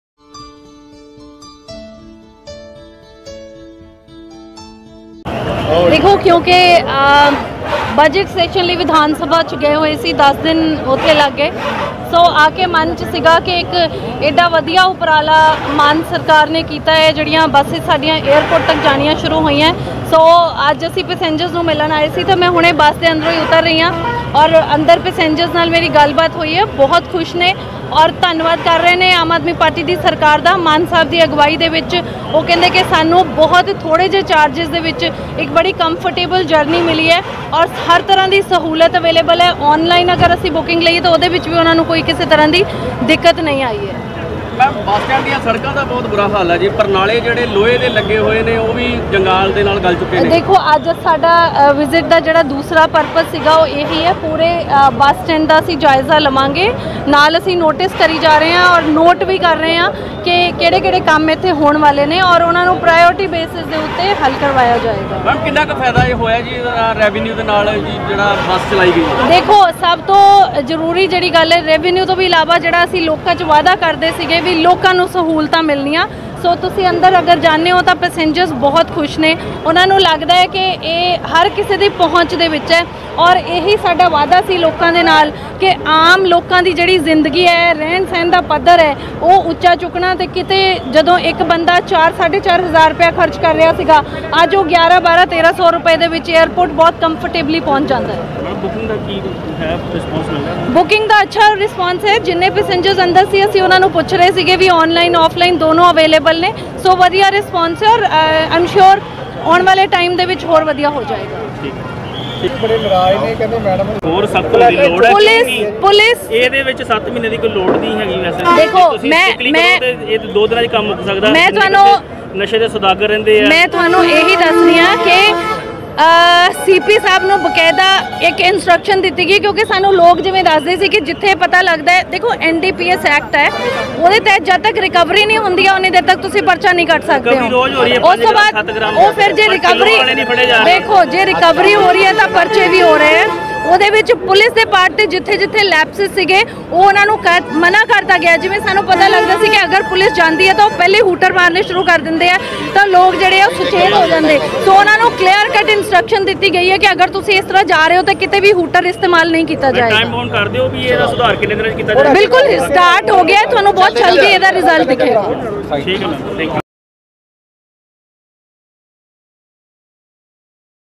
ਅੰਮ੍ਰਿਤਸਰ ਤੋਂ ਦਿੱਲੀ ਏਅਰਪੋਰਟ ਜਾਣ ਵਾਲੇ ਯਾਤਰੀਆਂ ਨਾਲ ਬੱਸ ਸਟੈਂਡ ਤੇ ਵਿਧਾਇਕ ਜੀਵਨਜੋਤ ਕੌਰ ਨੇ ਕੀਤੀ ਗੱਲਬਾਤ
ਵਿਧਾਨ ਸਭਾ ਹਲਕਾ ਪੂਰਬੀ ਦੀ ਵਿਧਾਇਕ ਜੀਵਨਜੋਤ ਕੌਰ ਨੇ ਅੰਮ੍ਰਿਤਸਰ ਦੇ ਬੱਸ ਸਟੈਂਡ ਤੇ ਕੀਤਾ ਦੌਰਾ